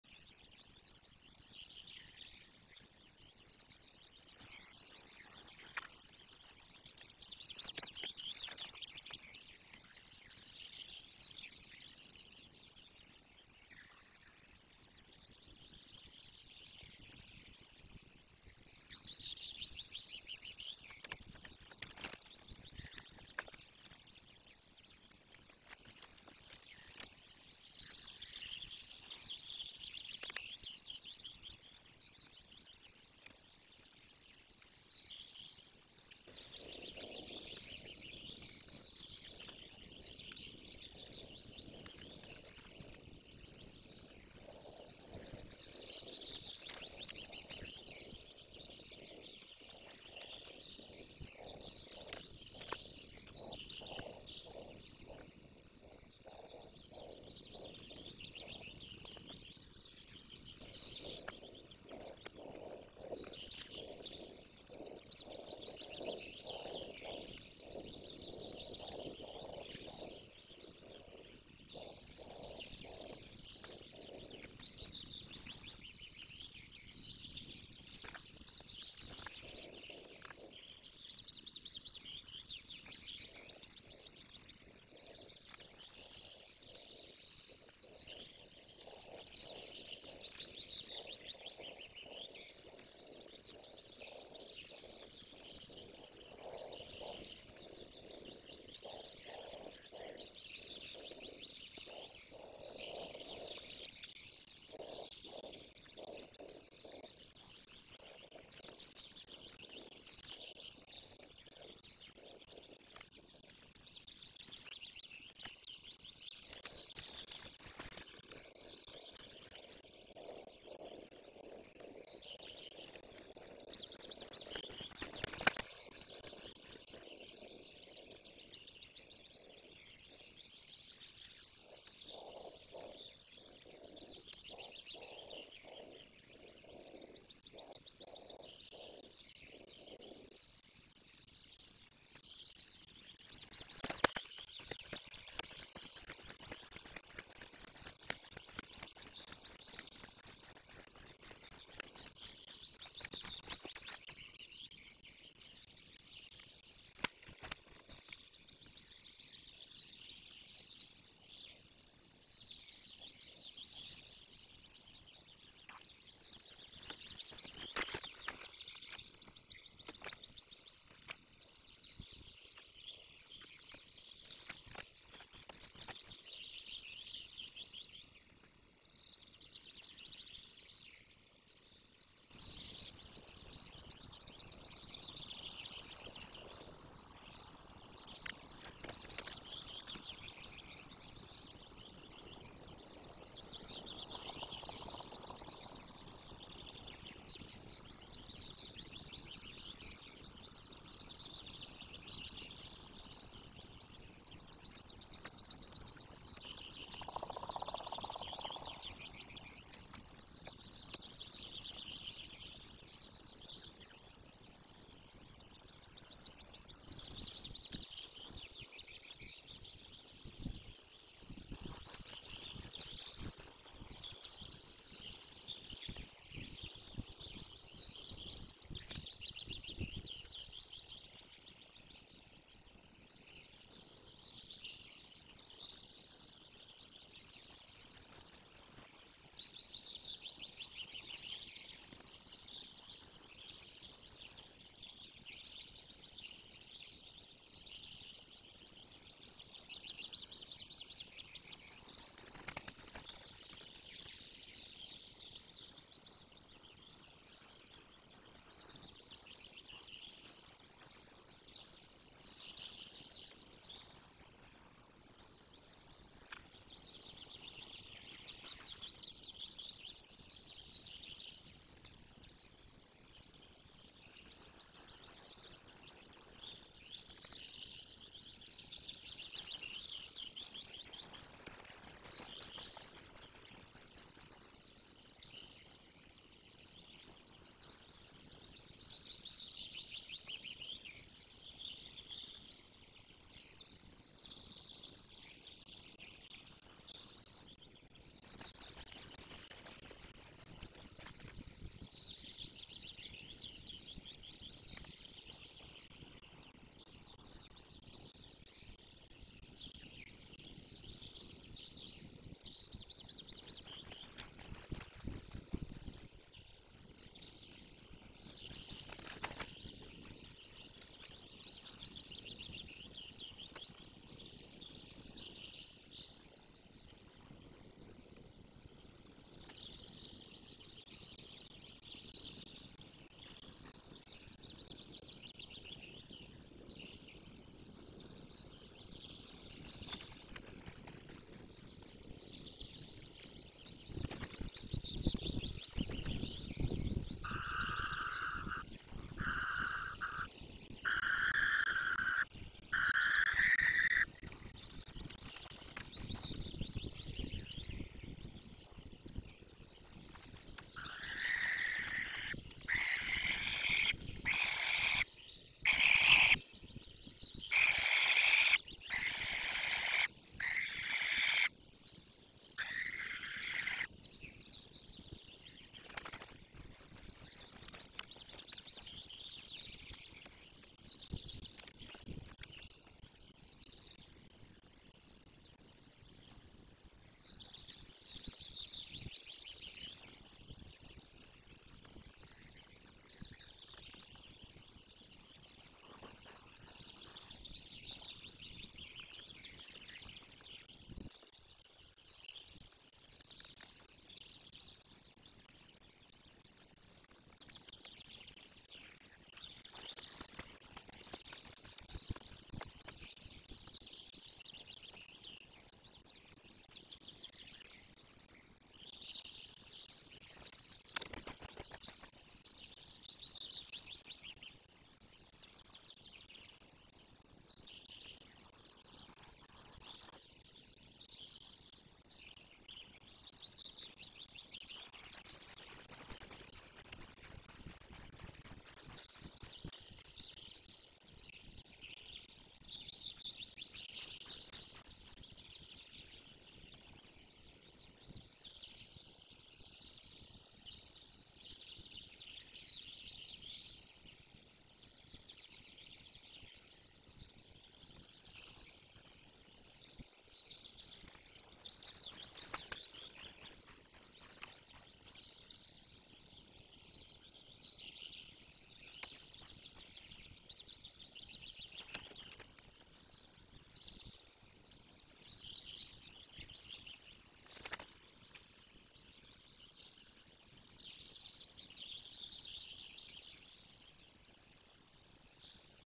Pierwszy lot - odlot z gniazda samczyk 6 EU 24 05 2024 Dolina Baryczy sokoły wędrowne gniazdo las
6 EU wystartował w swój  dziewiczy pierwszy lot młode sokoły  wędrowne opuszczają gniazdo
Poleciał Pierwszy odlot z gniazda samczyk 6 EU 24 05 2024 Dolina Baryczy sokoły wędrowne gniazdo las